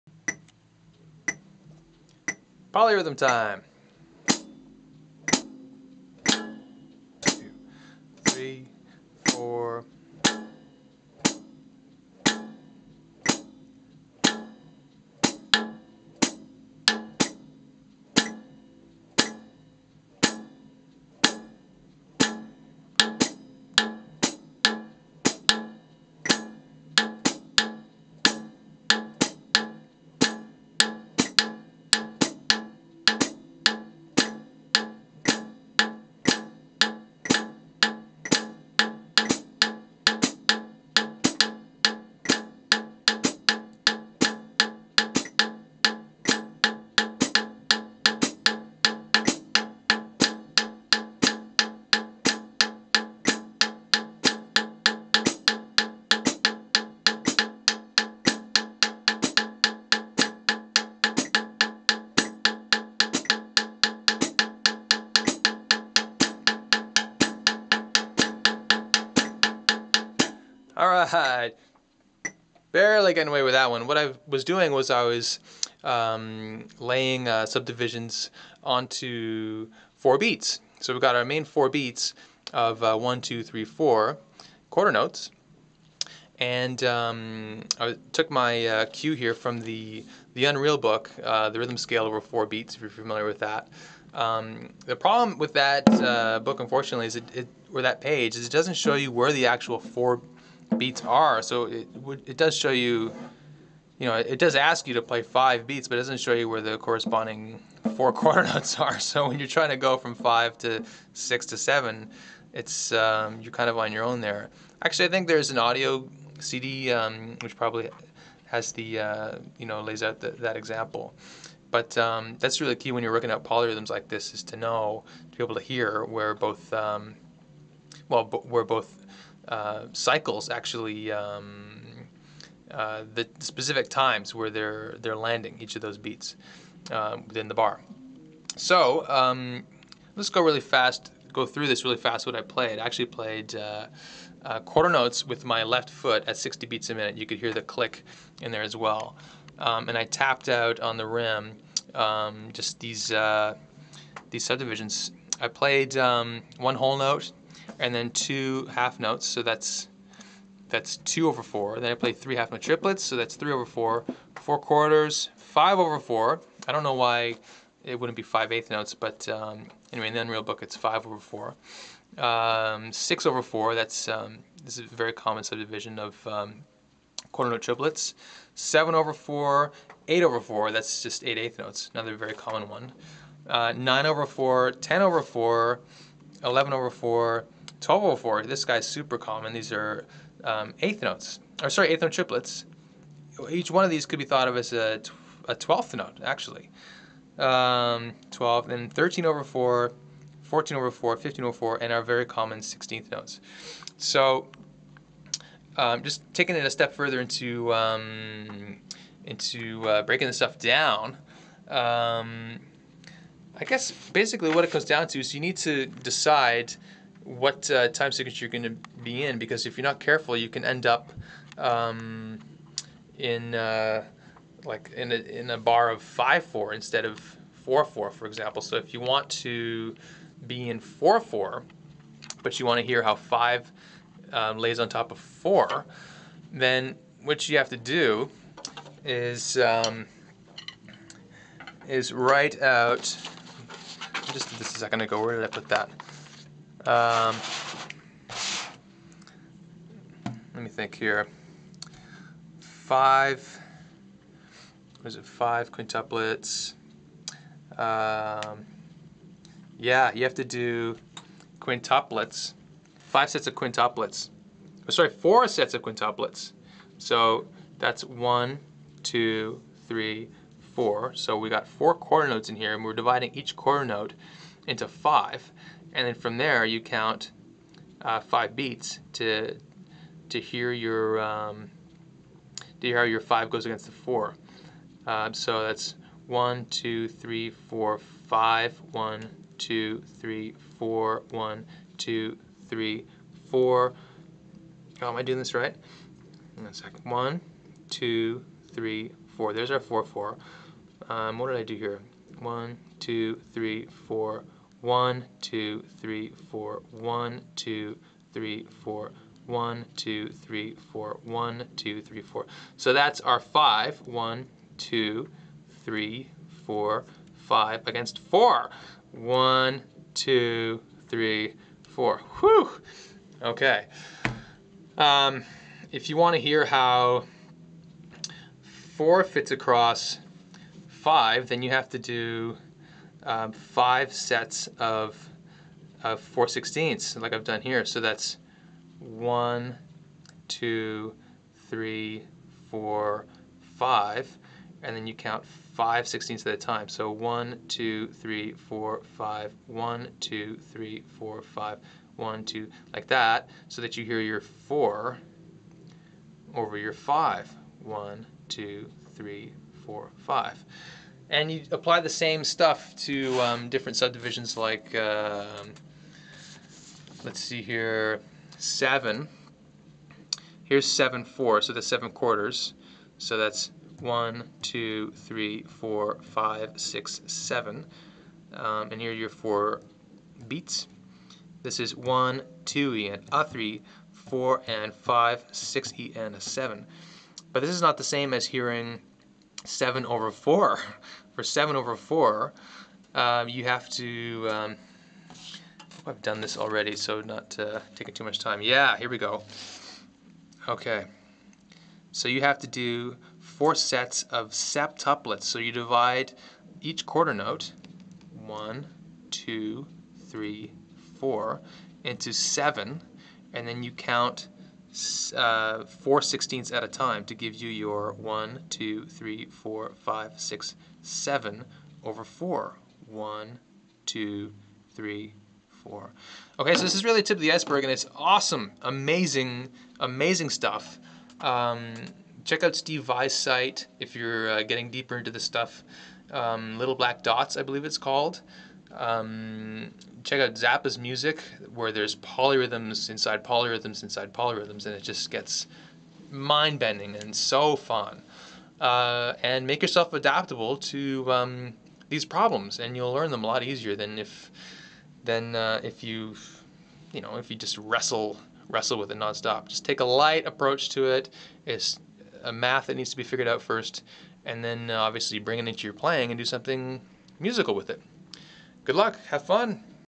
Adam Hay: An Intro to Polyrhythms and Subdivisions over Four Beats
This video features an intro to polyrhythms and subdivisions over four beats. Lots of fun! (And played almost correctly ... ha ha ha! The fives were a little off, as were some other sudivisions ... but who's counting?)
I played 1:4, 2:4, 3:4, 4:4, 5:4, 6:4, 7:4, 8:4, 9:4, 10:4, 11:4, 12:4, 13:4, 14:4, 15:4, and 16:4.
All one bar each (thus, entire scale is in 4/4).

The usual suspects are whole notes, half notes, half note triplets, quarter notes, quarter note triplets, eighth notes, eighth note triplets, and 16th notes.

Obviously this is tricky stuff, but it's lots of fun! Just make yourself adaptable to the math, understanding how the polyrhythms fit together, then get to work (patiently!) assembling it all. Good luck, go slow, and have fun! If it feels like a root canal, you haven't understood the math yet, that's all: do the math, then do the music.

Zappa and Steve Vai (littleblackdots.com) are the dudes to dig from here. You'll find polyrhythms within polyrhythms within polyrhythms and, yes, your head WILL explode ... (or your money back).